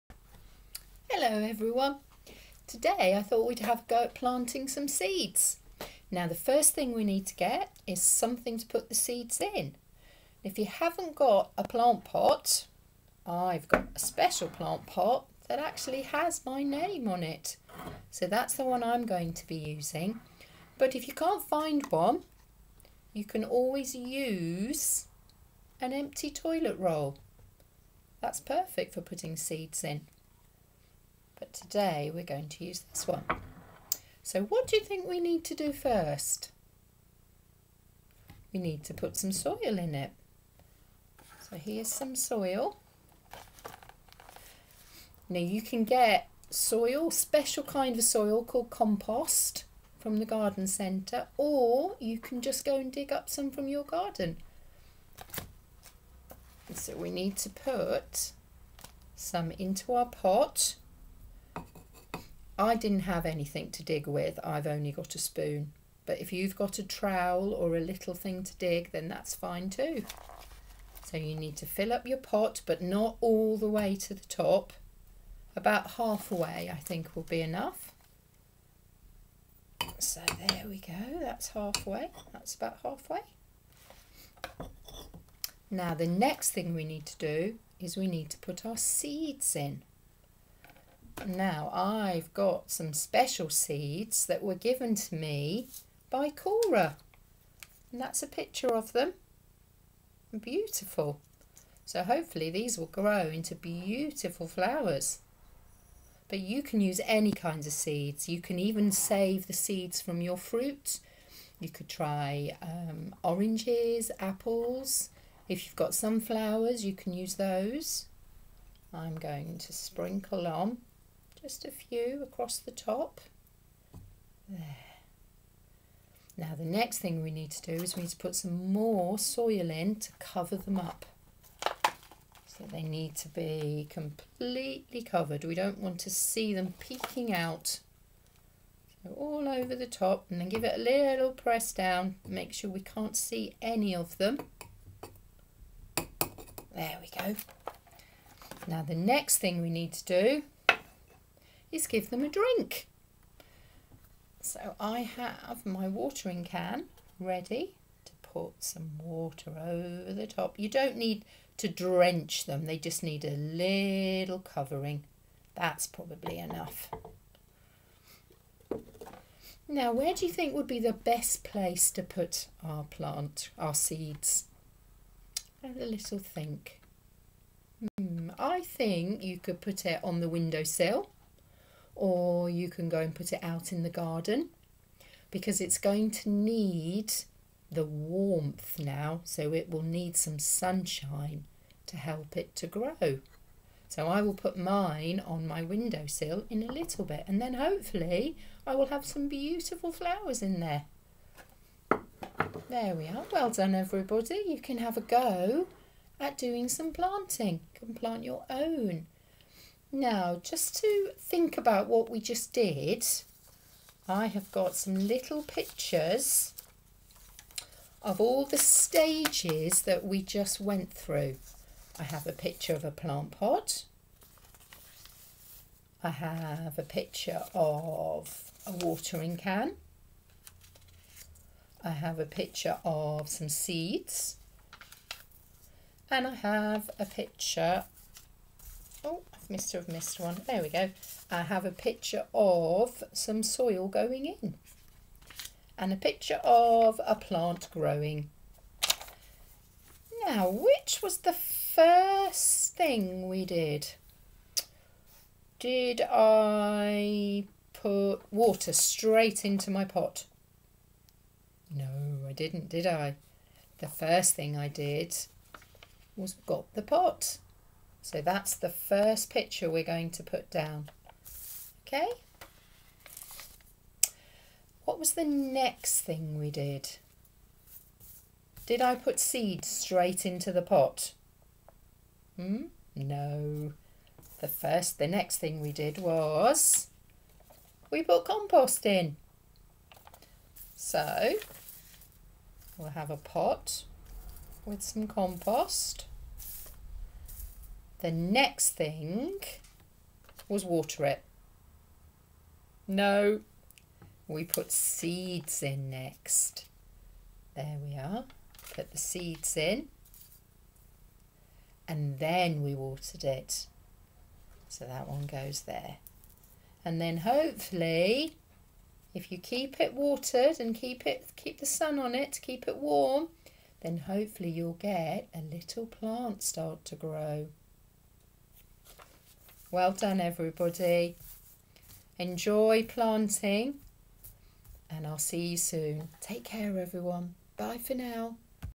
Claires Court Nursery - Planting Seeds
With Spring in full bloom, it's time to get green-fingered as Miss Green shows us how to plant seeds at home. To get started all you need is a pot, some soil and seeds.